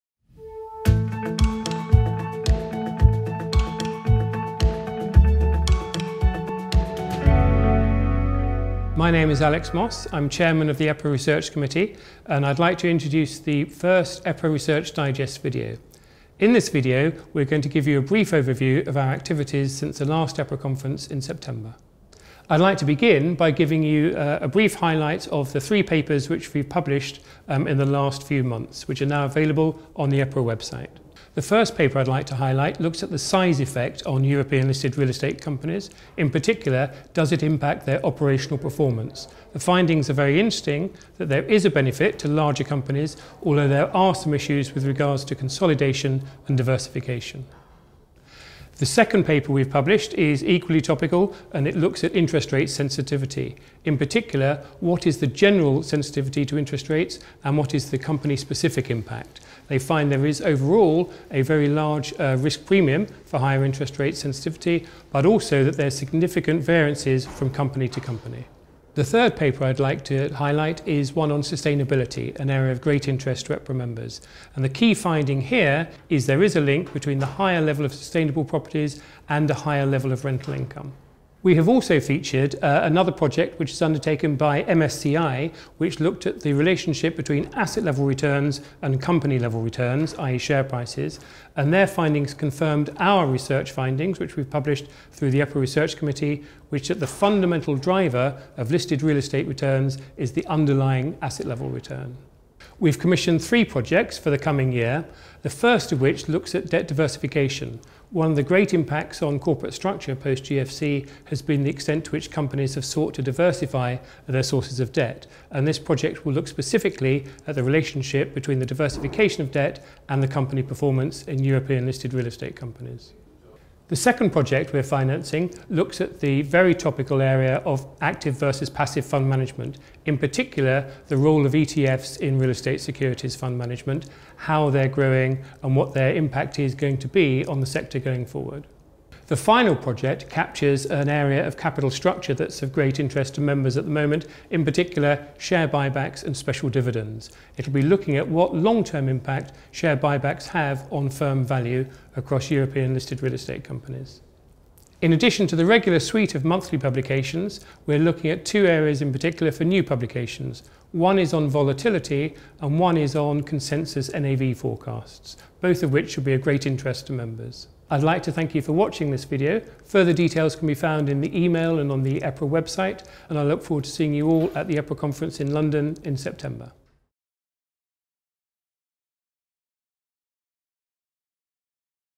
EPRA Research Digest - June 2017 - Introduction by Alex Moss
Welcome to the first edition of EPRA Research Digest.

At the Annual Conference in Paris last year we gave a commitment to ensure that the works commissioned by the Research Committee were commercially relevant, and that the results of the research were marketed clearly and effectively to EPRA members and other interested parties. This short video outlines the key points of the Research Digest.